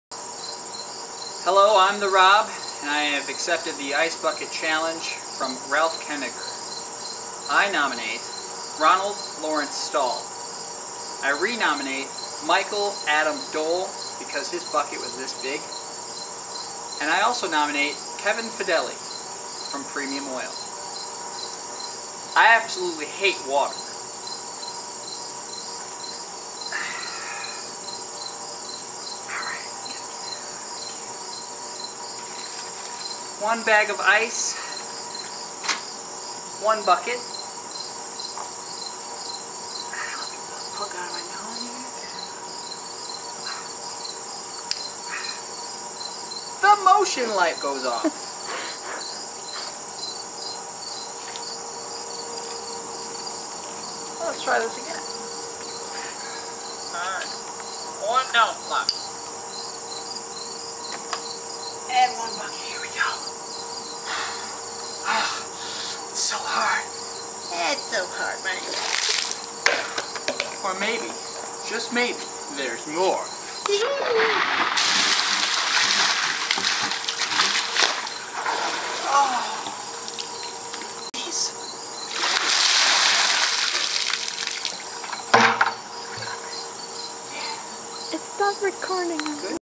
The ROB's Ice Bucket Challenge
Accepting the ALS Ice Bucket Challenge from Ralph Koeninger.

I challenge Ronald L Stahl, Michael A Dole, and Kevin Fedeli.
You have 24 hours to take the challenge or donate at ALSA.org